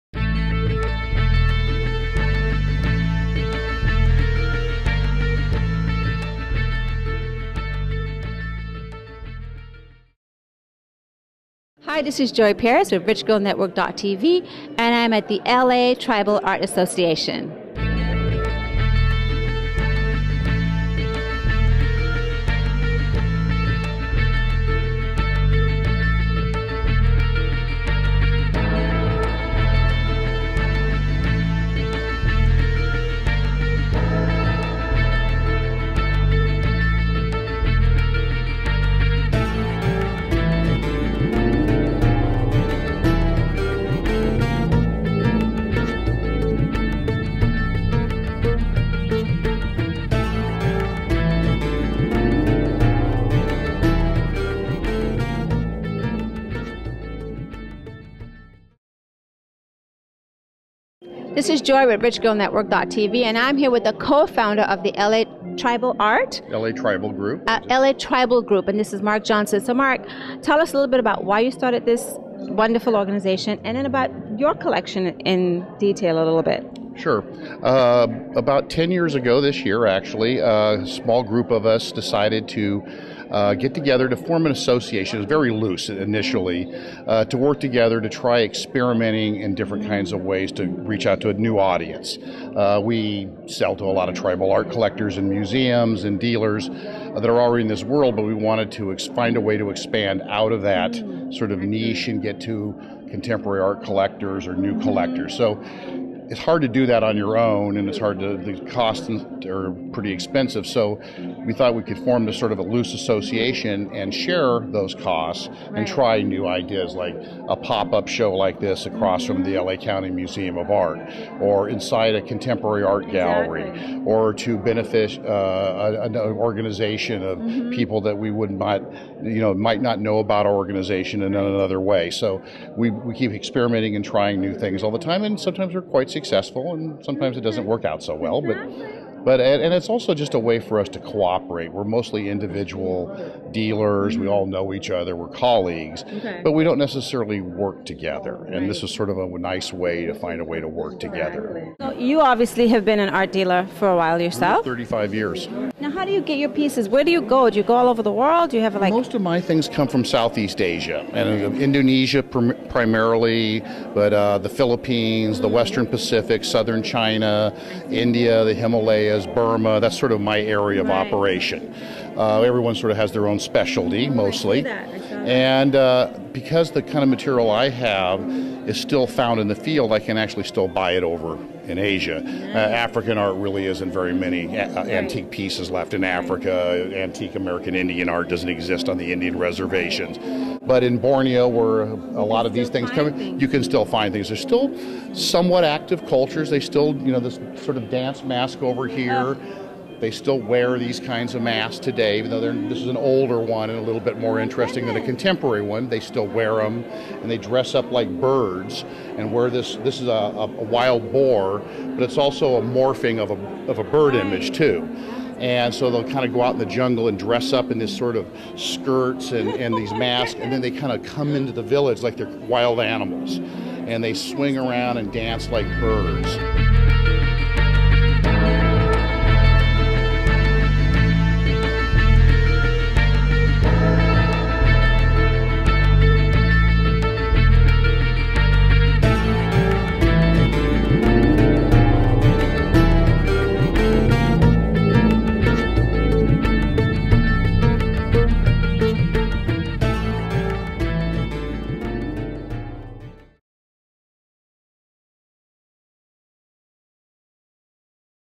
Co Founder of LA Tribal Group talks with Rich Girl
LA Tribal Group - an association of Tribal Art dealers, a true haven for art lovers and collectors.